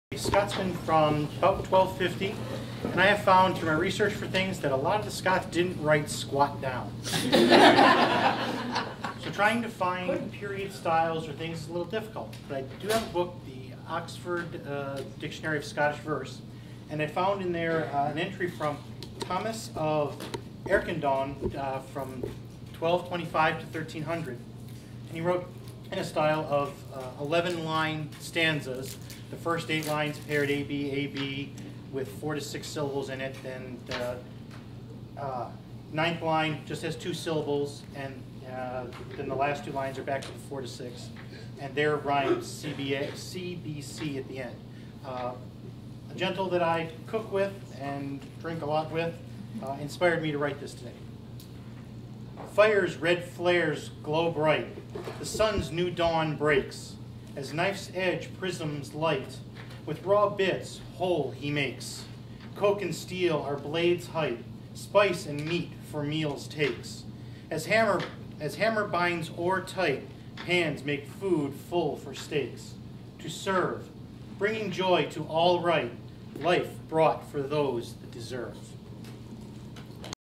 Scotish Verse
Alasdair Calum Montgomery answering the "Authenticity's Delight" challenge at Bardic Madness XX in the Barony of Windhaven on February 27, 2010.